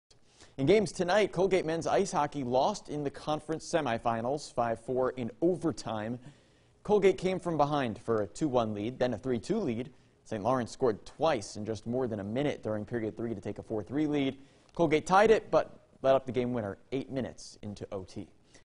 Colgate Men's Ice Hockey Falls in ECAC Semis
Eyewitness Sports Director David Edelstein - Colgate Men's Ice Hockey Falls in ECAC Semis